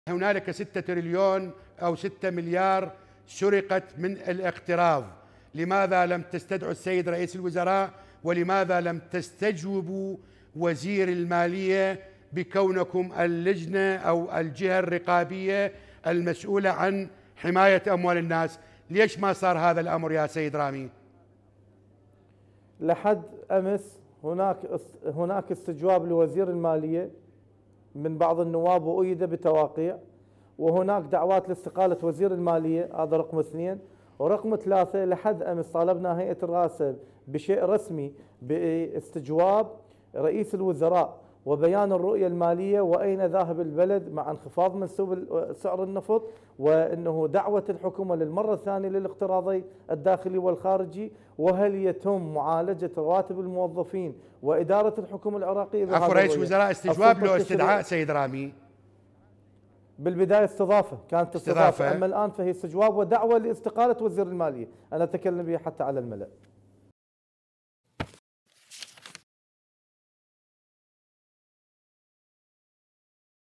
نجم الربيعي يكشف عن مصير 6 مليارات دينار مسروقة
لقاء نجم الربيعي مع النائب رامي السكيني ومحمود داغر | من بغداد